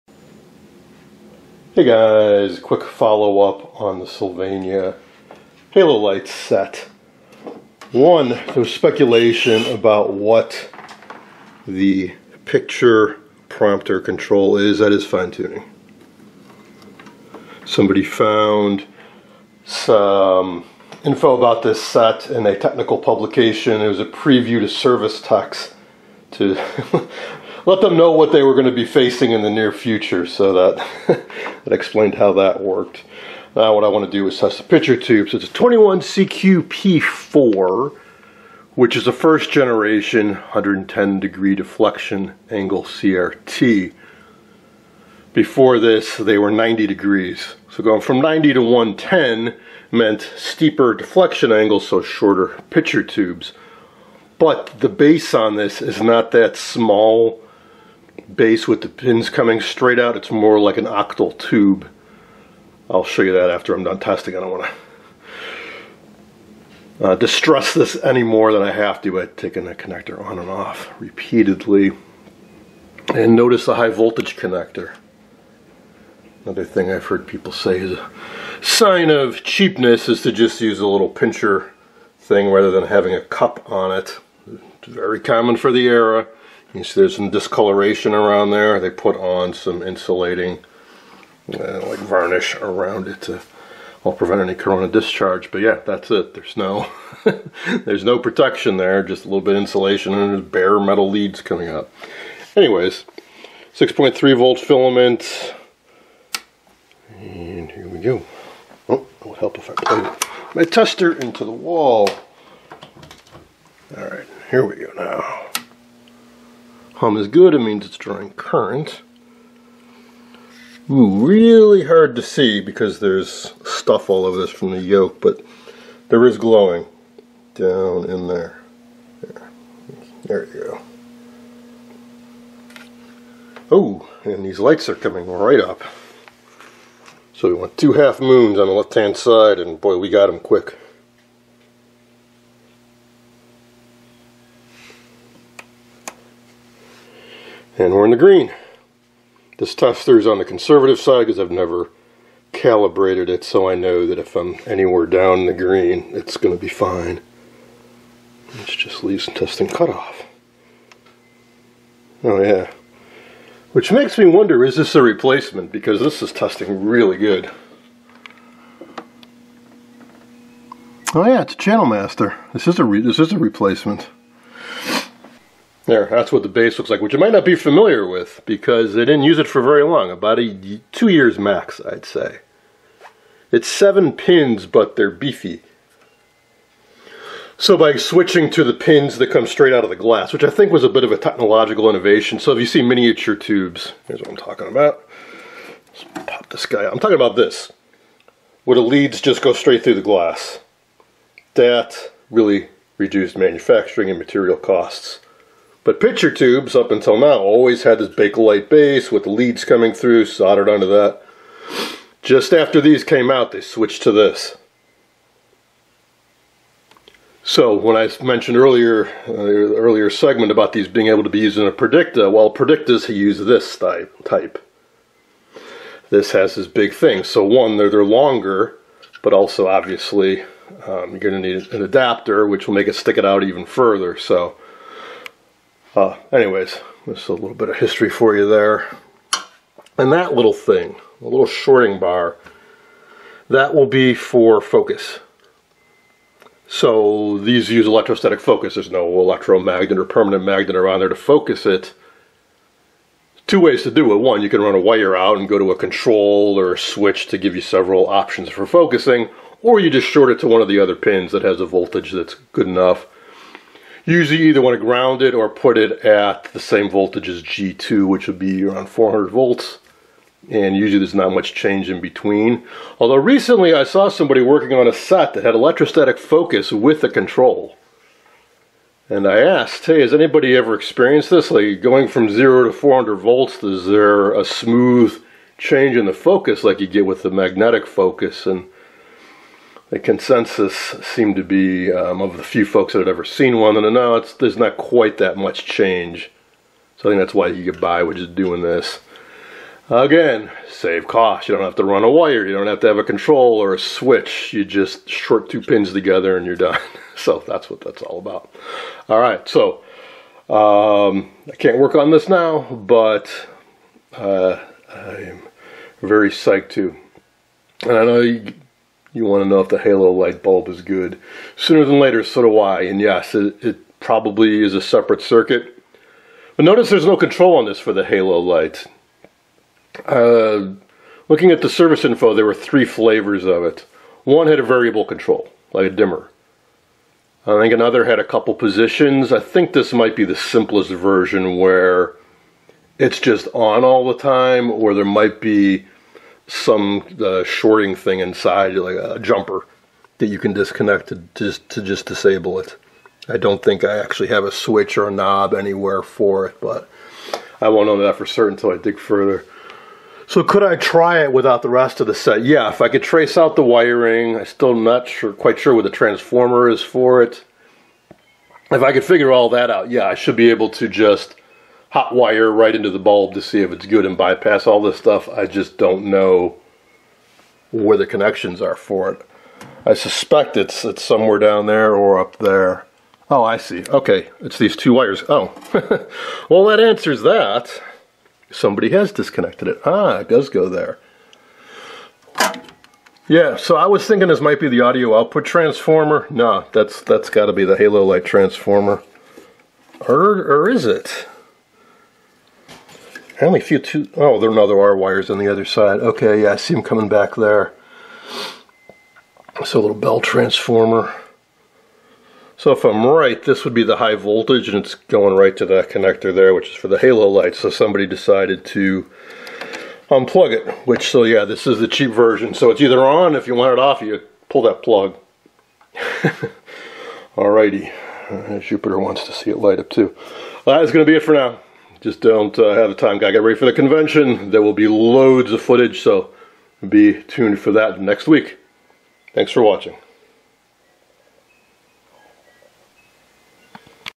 Sylvania Halolight/Sylouette 21C529 part 2
Testing the CRT and a bit more about the controls and halolight.
This channel is made possible by the generous support of viewers and Patrons.